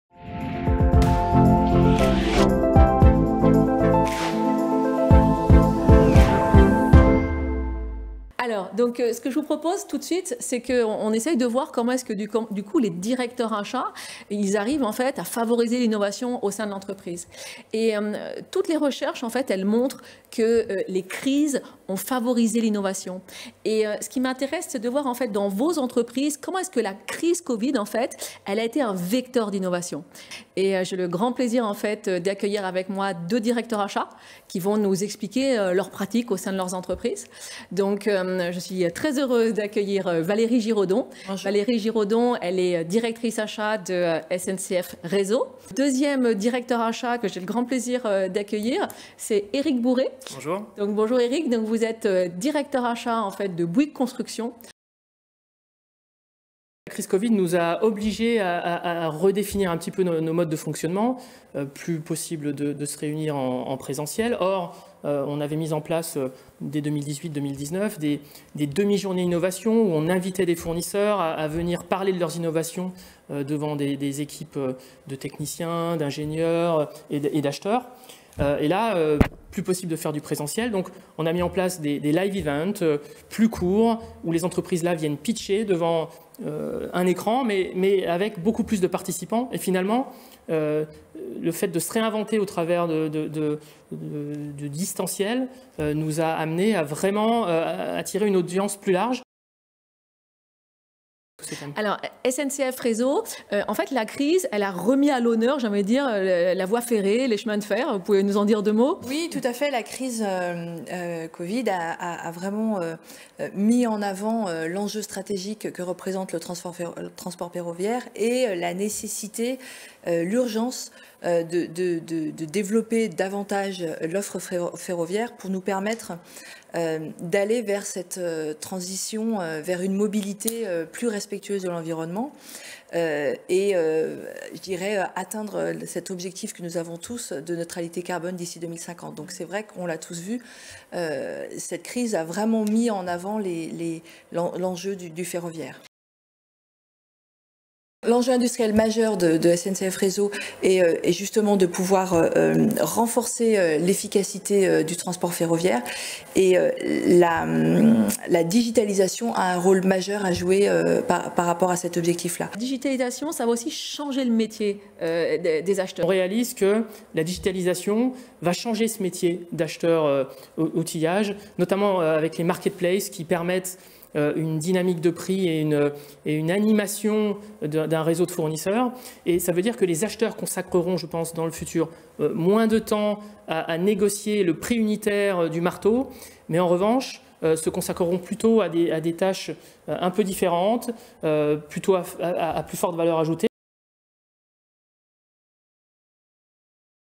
Crise et Innovation - Le rôle de la fonction Achats dans la RSE
Le principal obstacle à l’innovation est… le succès ! Les contraintes et les crises incitent à l’innovation ! La crise COVID en remettant en question l’avantage concurrentiel des entreprises, la solidité et résilience de leur Supply Chain, leurs relations avec leurs clients et leurs fournisseurs… s’avère être un formidable accélérateur d’innovations.

Comment les CPO contribuent-ils à cette dynamique d’innovation ? Comment appréhendent-ils le renouveau des relations clients-fournisseurs ?
Cette crise a aussi démontré la puissance du collectif.
Comment les CPO vont-ils continuer à favoriser l’intelligence collective entre l’entreprise et ses fournisseurs ?
Comment comptent-ils sourcer de nouvelles approches innovantes ?

Le Dr. Natacha Tréhan a animé cette table ronde avec :
- Valérie Giraudon, CPO chez SNCF Réseau
- Eric Bouret, CPO chez Bouygues Construction